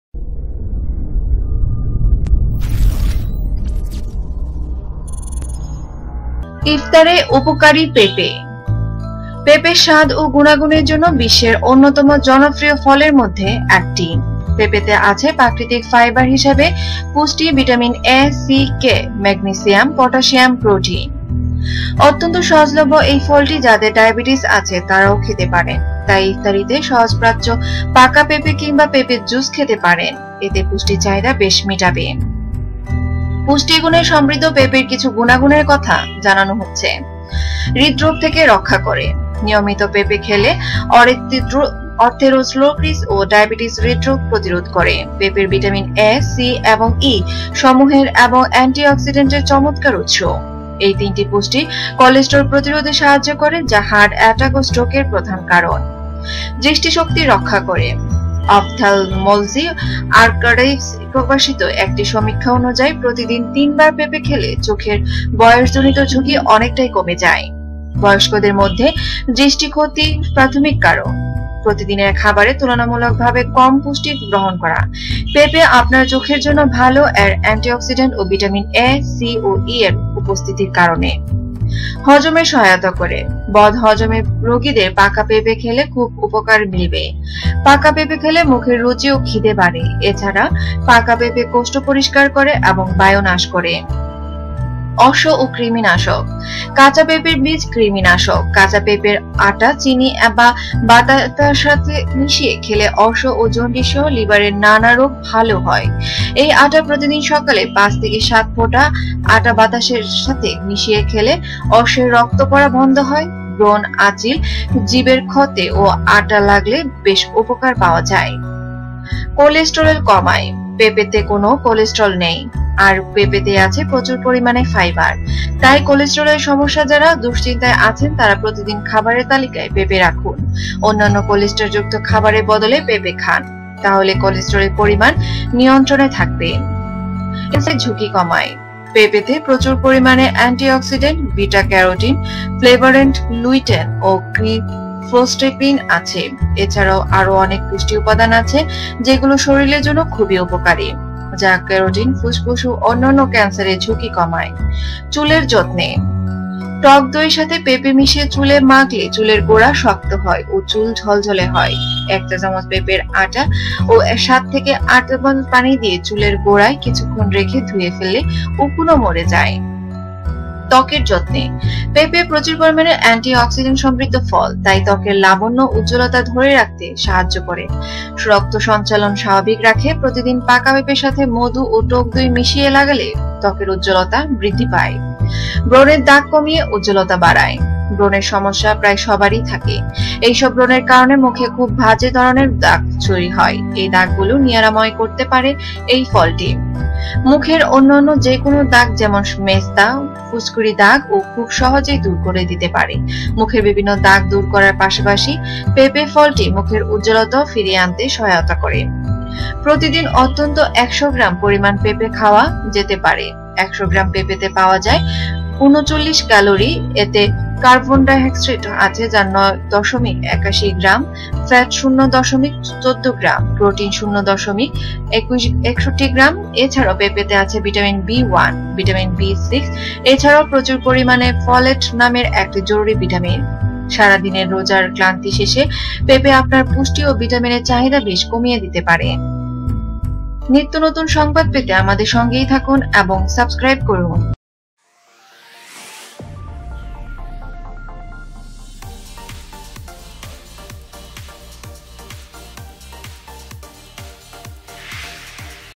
ইফতারে পেঁপের উপকারিতা
নিয়মিত পেঁপে খেলে অথেরোস্ক্লেরোসিস এবং ডায়াবেটিক হৃদরোগ প্রতিরোধ করে। পেঁপের ভিটামিন এ, সি এবং ই, সমূহের এবং অ্যান্টিঅক্সিডেন্ট এর চমৎকার উৎস।

এই তিনটি পুষ্টি কোলস্টেরল প্রতিরোধে সাহায্য করে, যা হার্ট এটাক ও স্ট্রোক এর প্রধান কারণ।

দৃষ্টিশক্তি রক্ষা করে:

অপথ্যালমোলজি আর্কাইভস প্রকাশিত একটি সমীক্ষা অনুযায়ী, প্রতিদিন তিনবার পেঁপে খেলে চোখের বয়সজনিত ঝুঁকি অনেকটাই কমে যায়। বয়স্কদের মধ্যে দৃষ্টি ক্ষতি প্রাথমিক কারণ, প্রতিদিনের খাবারে তলনামূলক ভাবে কম পুষ্টি গ্রহণ করা। পেঁপে আপনার চোখের জন্য ভাল এর অ্যান্টিঅক্সিডেন্ট ও ভিটামিন এ, সি, ও ই এর উপস্থিতির কারণে।